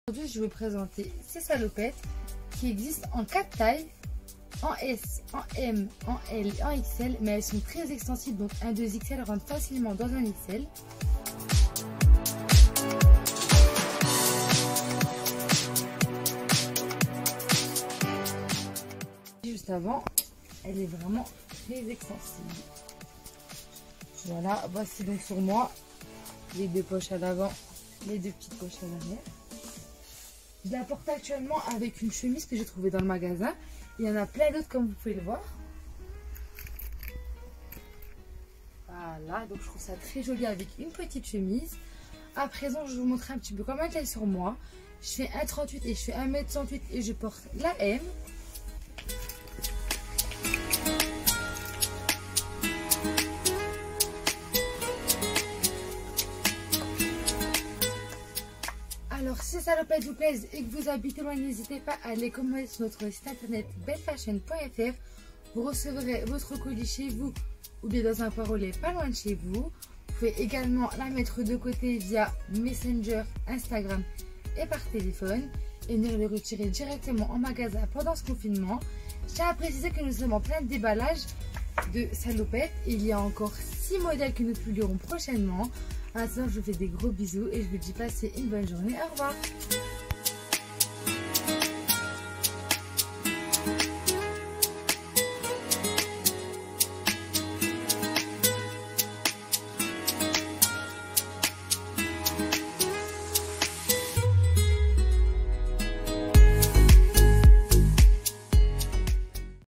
Salopette Jean coupe ample à bretelles
Salopette Jean coupe ample, 4 boutons devant, bretelles ajustables, 2 poches latérales et 2 poches derrières. Tailles: S, M, L, XL. Prenez une taille de moins que votre taille habituelle.  Taille S sur la mannequin.